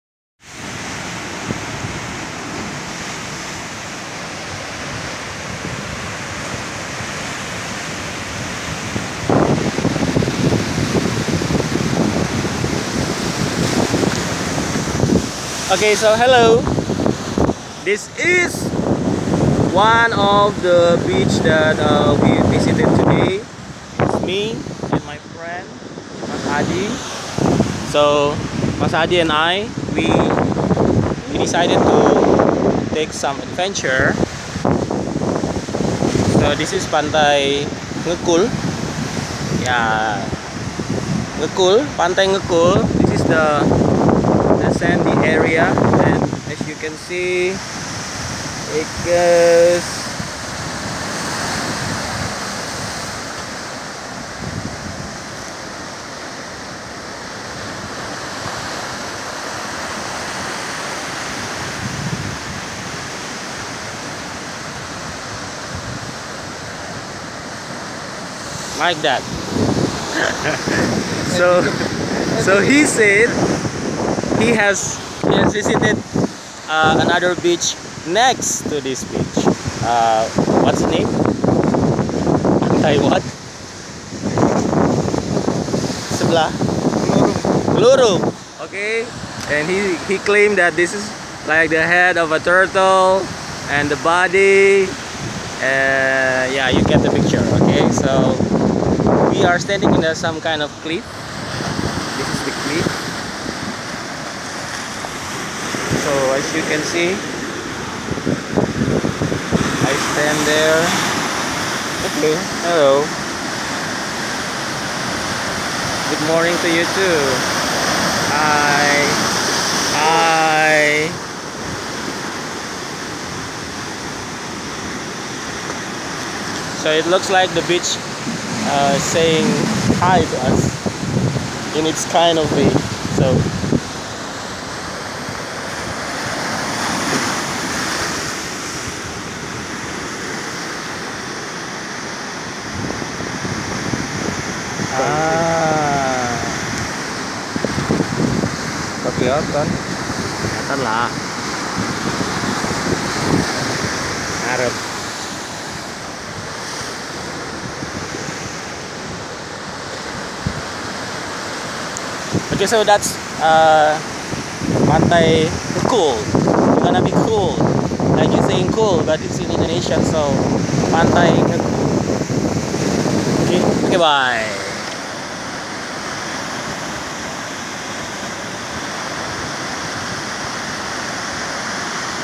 ADVENTURE TIME - NGEKUL BEACH, SOUTH MALANG
It's beach time! "Pantai" is an Indonesian word translated as beach. Enjoy!